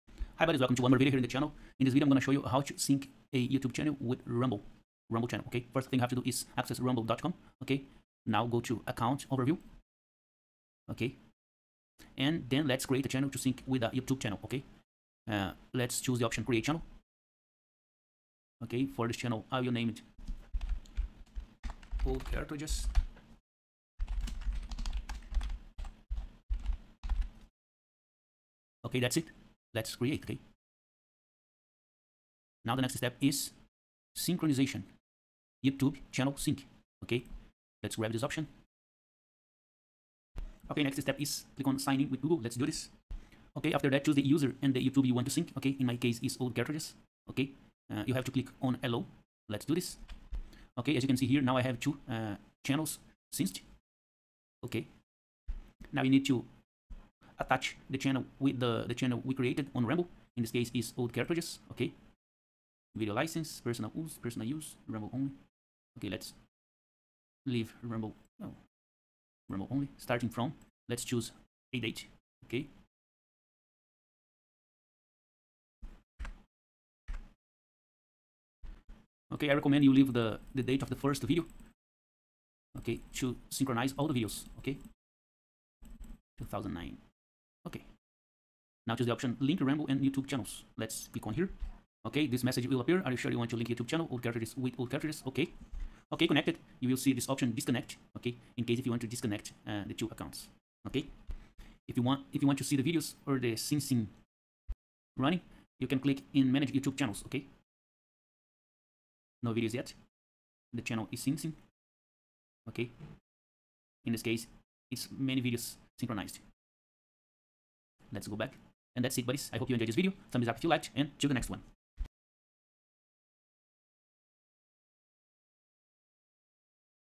How to sync YouTube Channel on Rumble video Platform
In this video, I show you how to synchronize YouTube videos with the Rumble video Platform. This is a great idea if you want to back up your YouTube vídeos with a minimum effort.